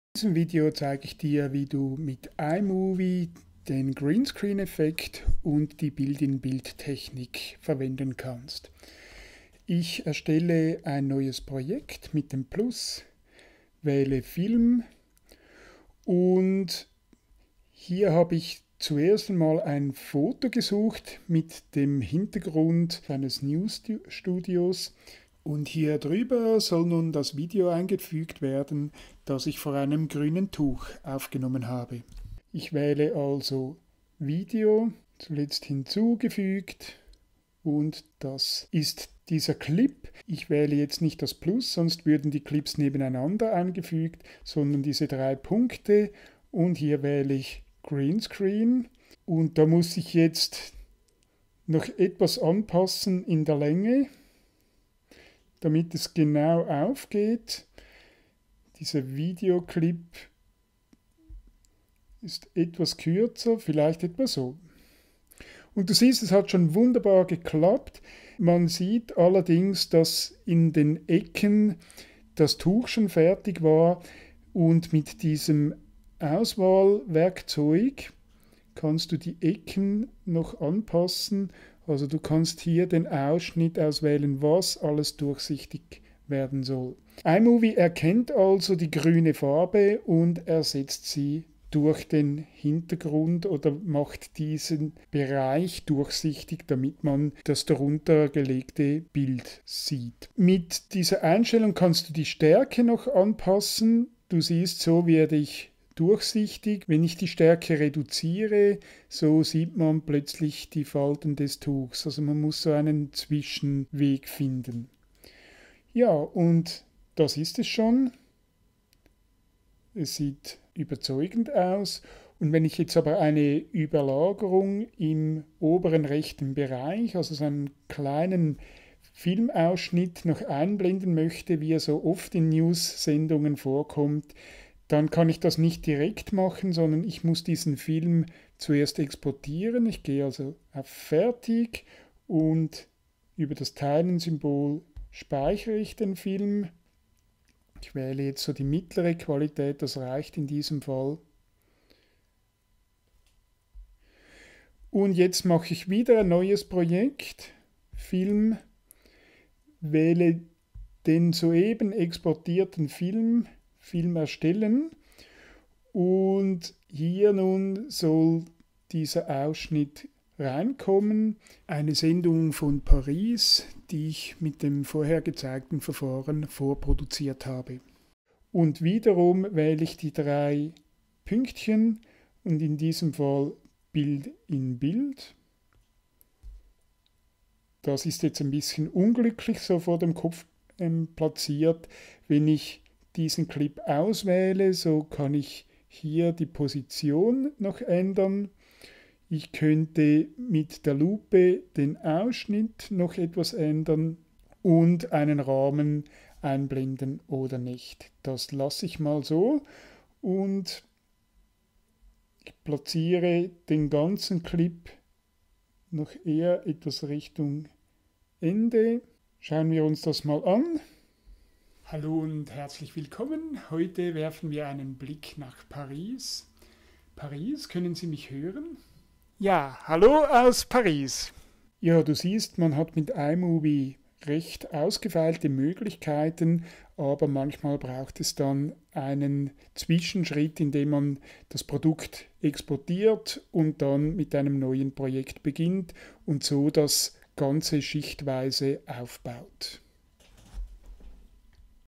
iMovie für iPad: Greenscreen und Bild-in-Bild
Tutorial für die Videoeffekte mit Greenscreen und Bild-in-Bild